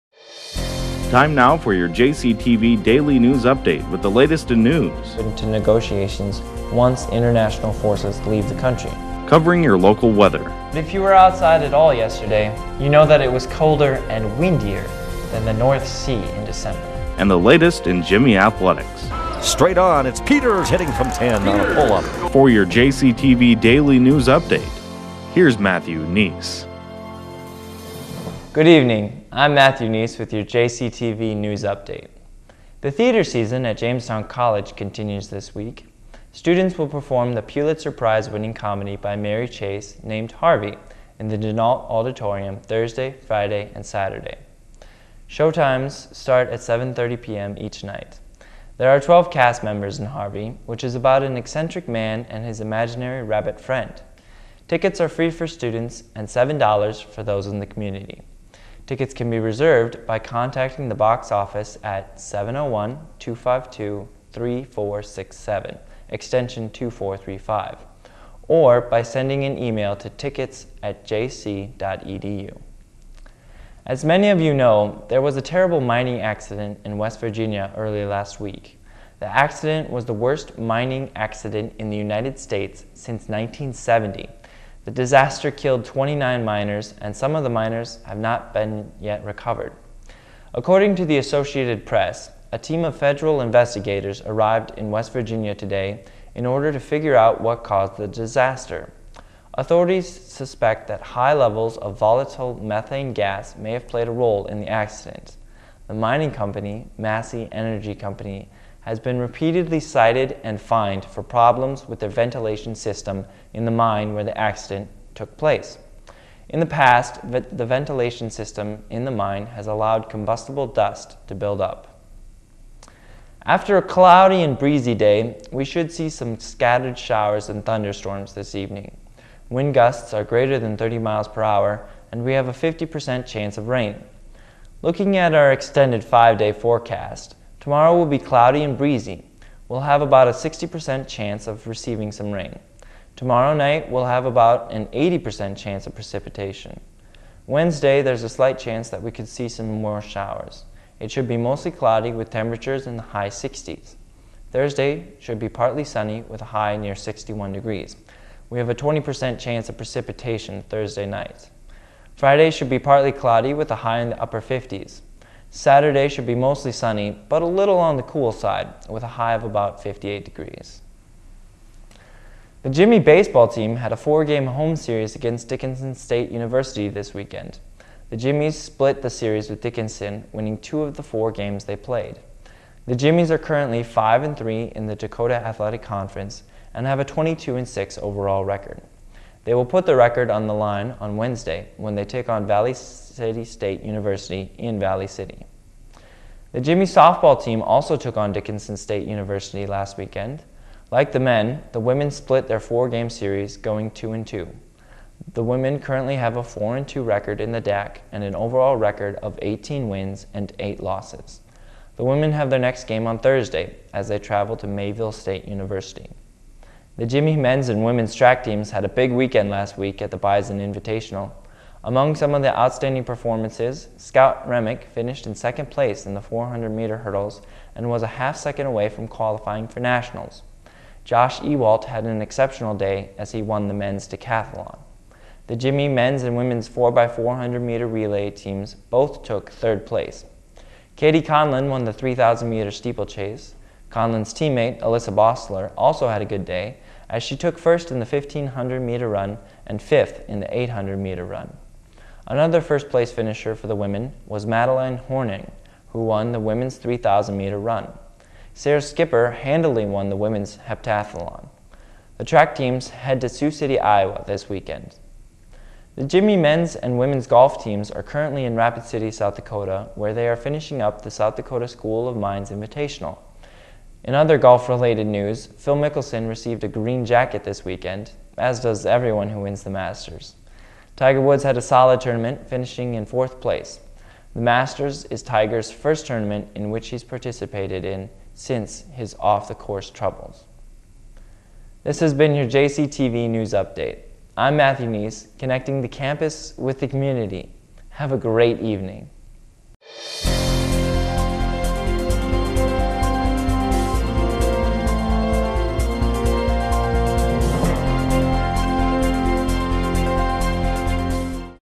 JCNU April 12
Matthew Nies delivers Jamestown, North Dakota's Monday evening news.